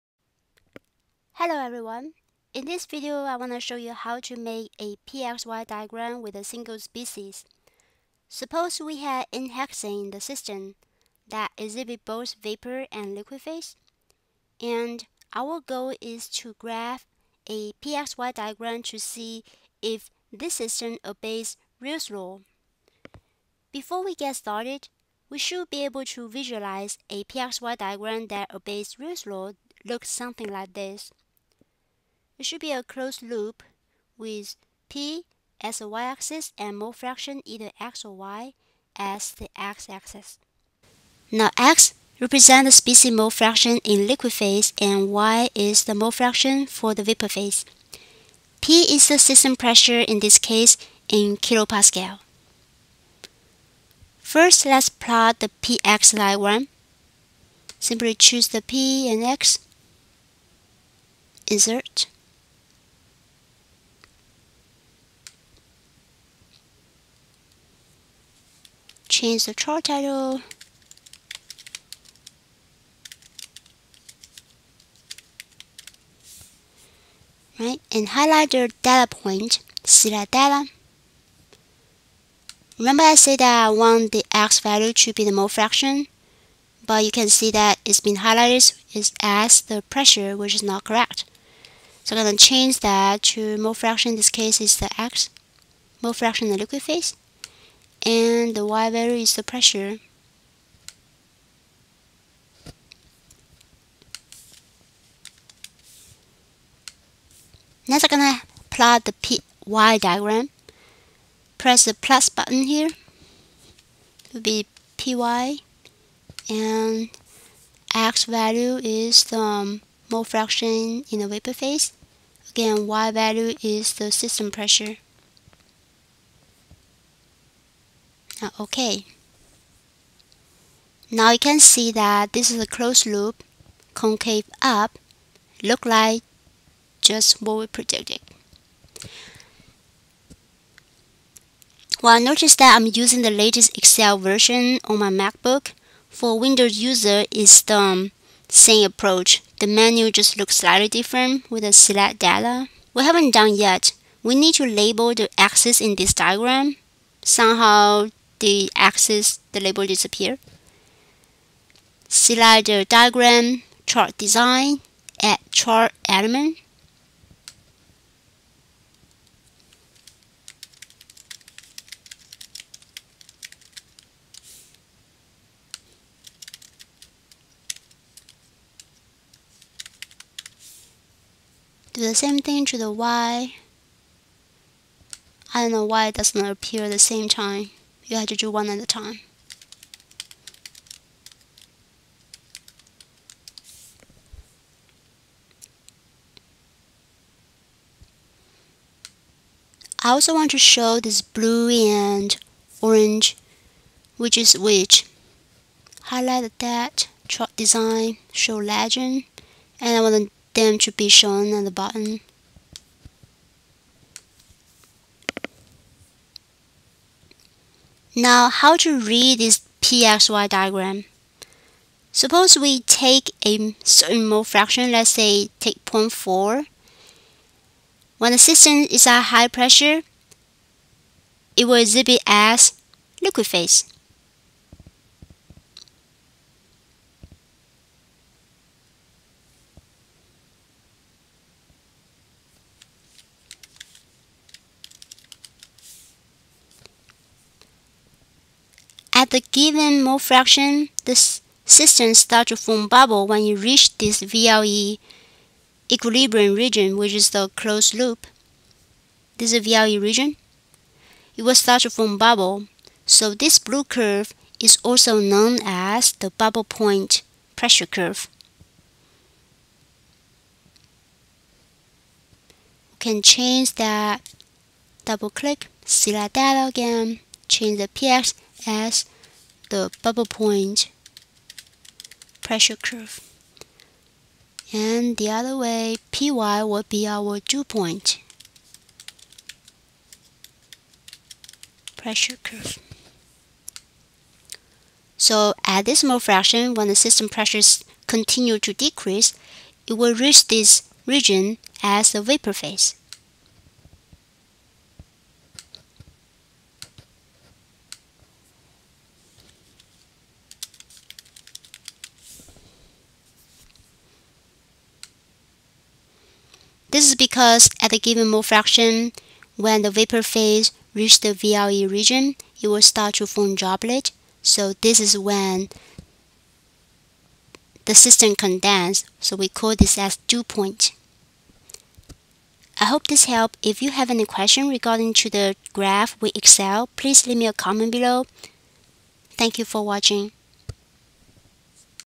How to Plot Pxy Diagram How to Plot Mutiple Data Sets On the Same Graph with Excel 2019
I'm using MS Excel latest version in 2019 on my MacBook to make this video. For Windows users, the menu looks slightly different, but it's the same approach.